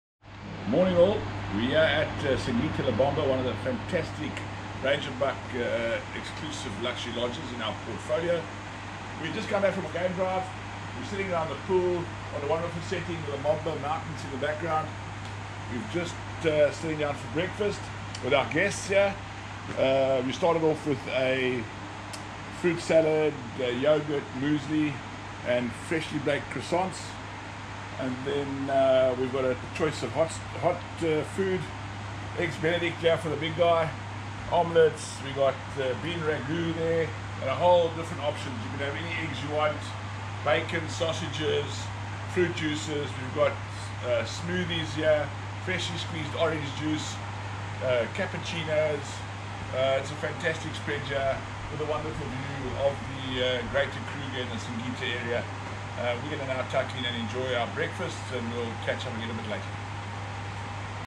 Breakfast With A View - Singita Lebombo Safari Lodge
"Break away to a brilliantly bold and contemporary space, suspended aloft the N’wanetsi River in the Kruger National Park.

The design of Lebombo Lodge was inspired by the many eagles’ nests built into the cliff face of the river bank, resulting in organic, light-filled structures, floating between river and sky."